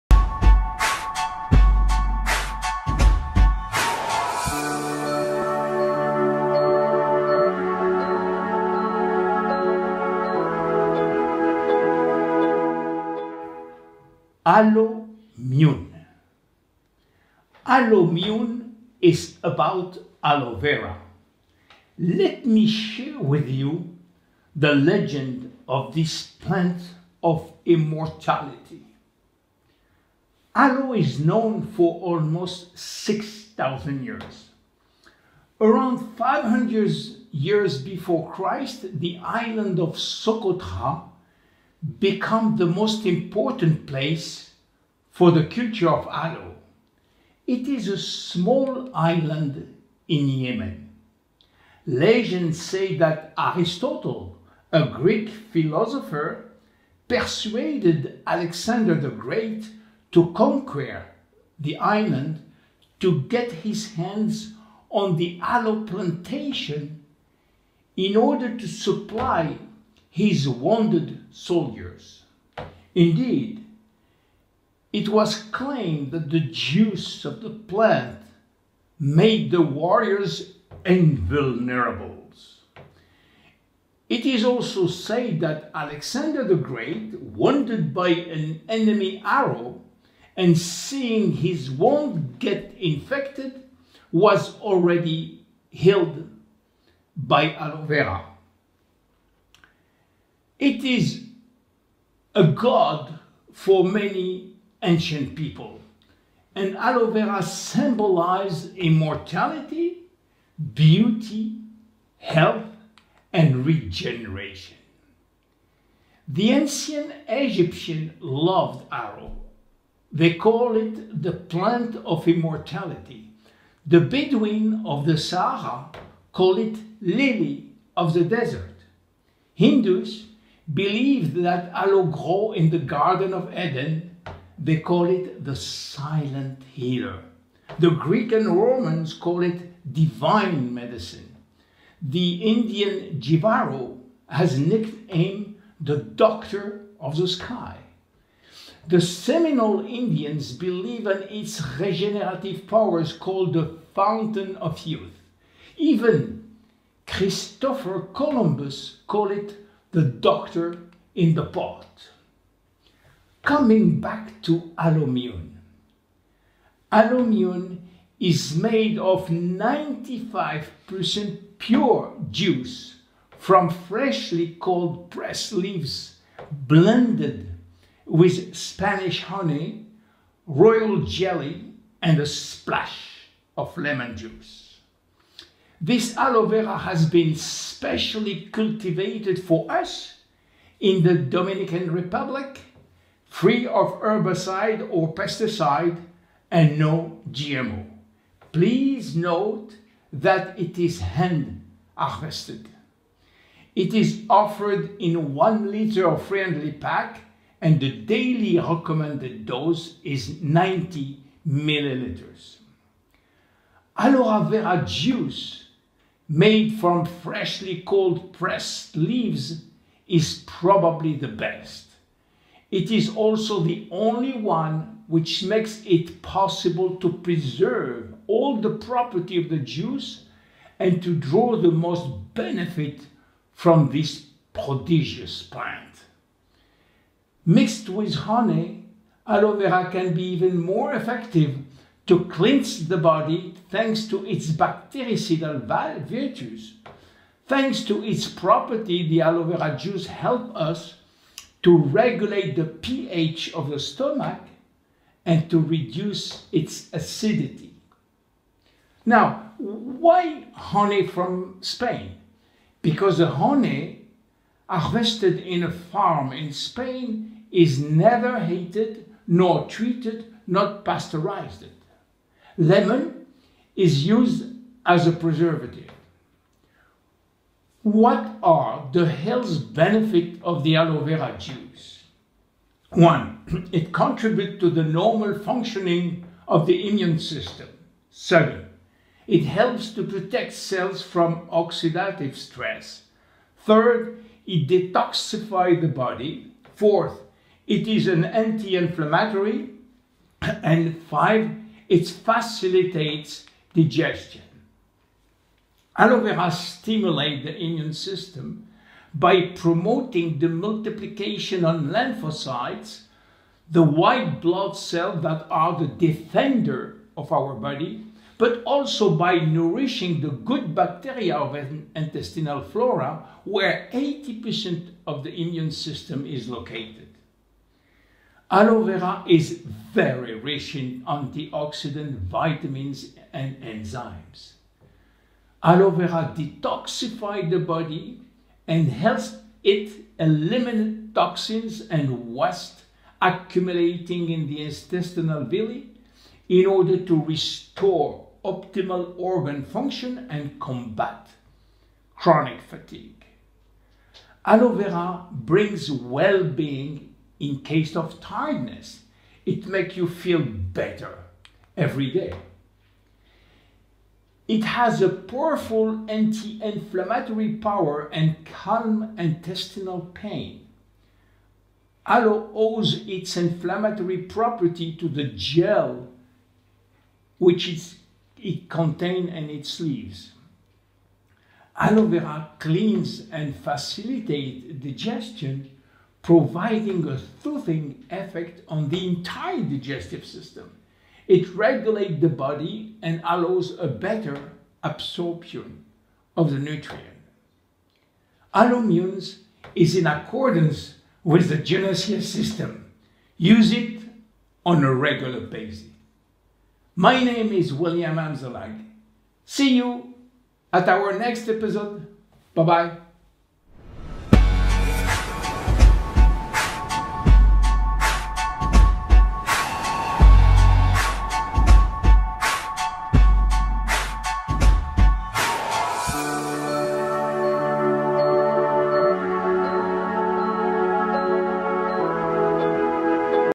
Episódio N 42: ALOMUN: ALOE VERA - A PLANTA DA IMORTALIDADE
Episódio N 42:  ALOMUN: ALOE VERA  - A PLANTA DA IMORTALIDADE                                          O Aloe é conhecido há quase 6.000 Anos                                                       Benefícios Incríveis                                                                                                            👉 Antioxidante                                                                                                                   👉 Cicatrizante                                                                                                                        👉 Anti-inflamatório                                                                                                                 👉 Antiviral                                                                                                                              👉 Antibacteriano                                                                                                                  👉 Antifúngico                                                                                                                      👉 Digestivo                                                                                                                          👉 Antiparasitário                                                                                                               👉 Desintoxicante                                                                                                                Legendas em Portugês 🧬✨🧭